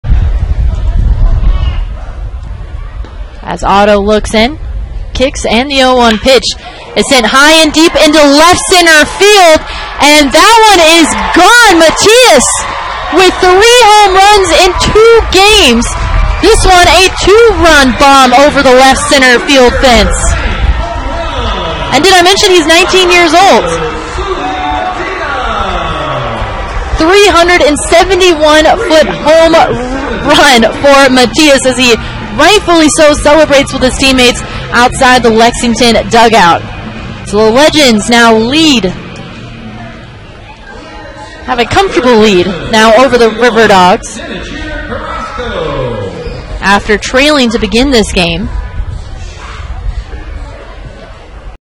Lexington's Matias seals 5-2 win with two-run homer.
4/6/18: Seuly Matias gives the Lexington Legends some insurance with a two-run homer in their 5-2 win over Charleston.